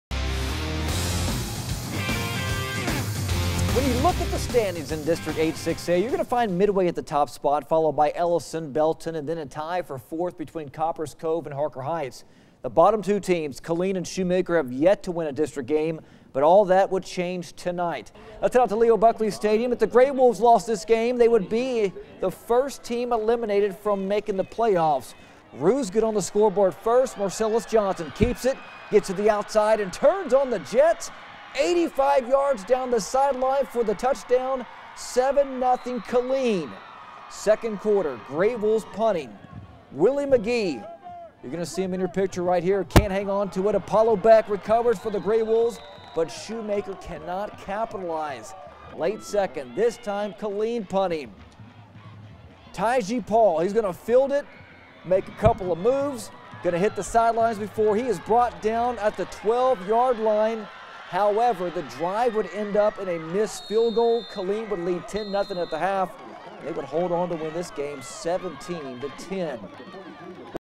Killeen, Shoemaker clash at Leo Buckley
When you look at the standings in District 8-6A, you'll find Midway at the top spot, followed by Ellison, Belton and then a tie for fourth between Copperas Cove and Harker Heights.